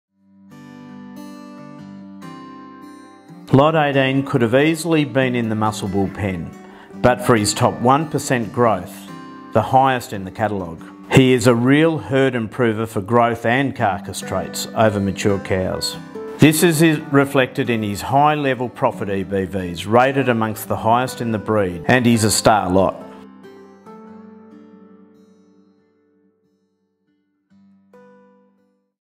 Lot 18 WNAT320 Wirruna Autumn Sale 2024
Wirruna Poll Herefords Autumn Bull & Female Sale.
On-Property using the Helmsman buying system from 1pm and interfaced with AuctionsPlus, Holbrook NSW.
74 Poll Hereford Bulls, 11 PTIC Breeders & 66 PTIC Breeders as group lots on offer.